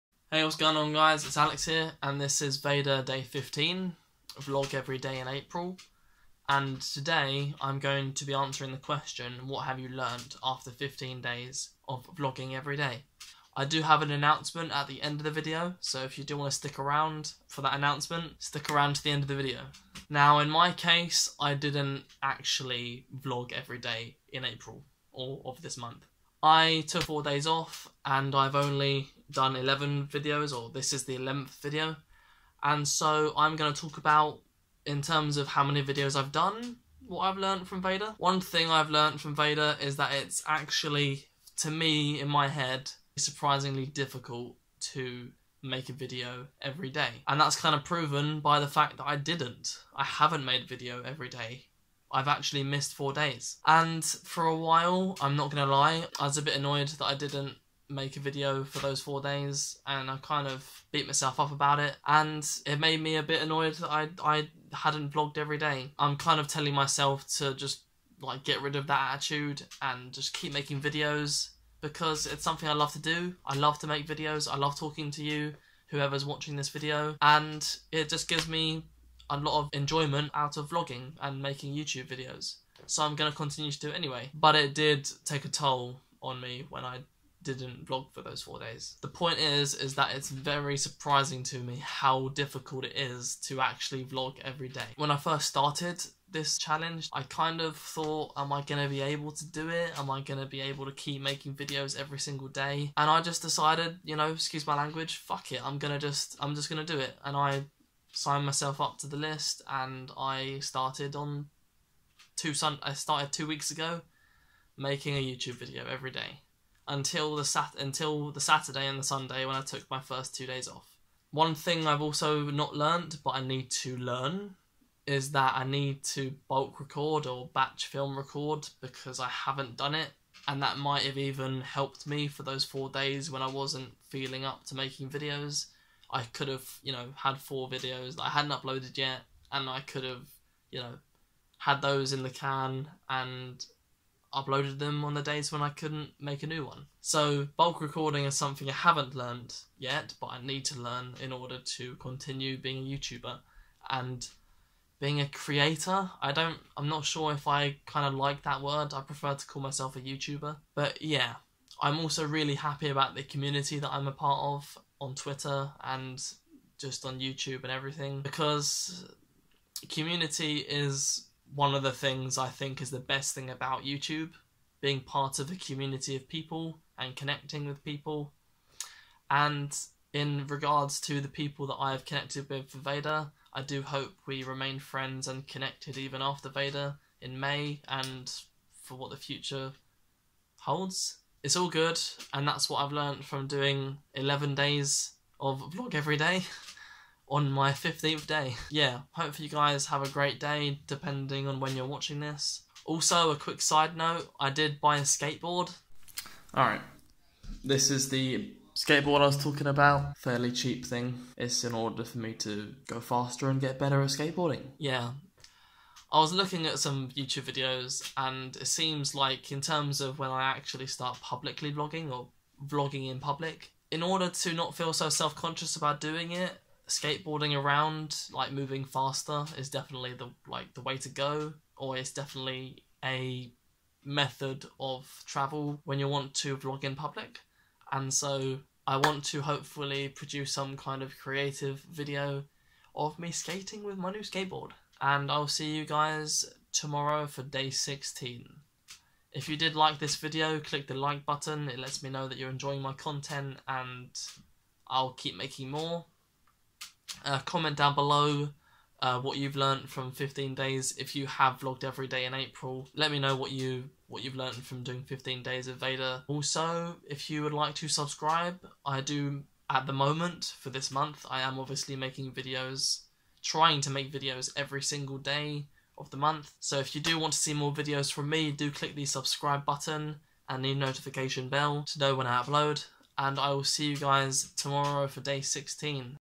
SSSVEDA 15 - What Have You Learned From 15 Days Of Vlogging
Welcome back to SSSVEDA 2018! Today I talk about what I've learnt and got out of vlogging (nearly every day) of April